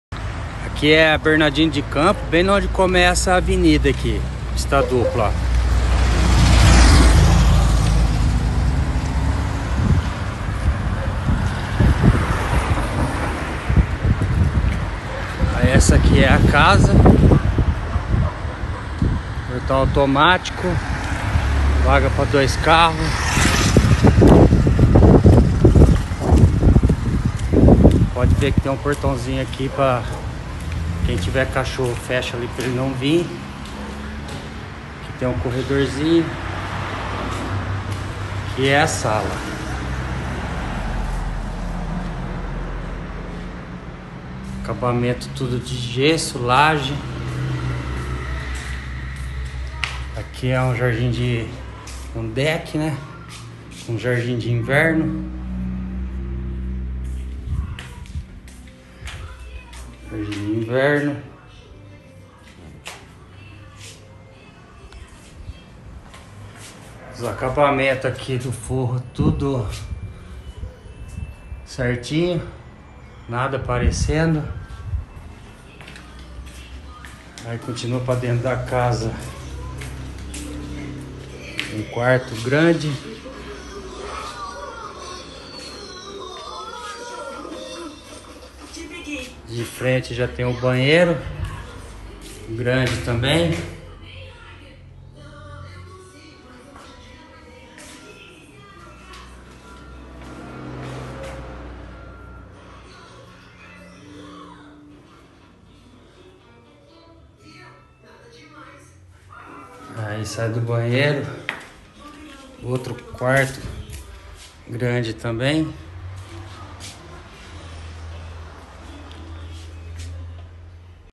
Vende-se casa localizada na Bernardino de Campos!!
MEDIDA: 160 M² CONSTRUÍDOS

A CASA POSSUI:

2 Quartos
1 Banheiro
1 sala de estar e sala de jantar
Jardim de inverno
Cozinha
Edícula com mais um quarto e banheiro e lavanderia.
Garagem com quatro vagas de carros sendo apenas duas cobertas.
Portão automático

ACEITA PERMUTA EM CARRO.

Não perca tempo, agende uma visita conosco!!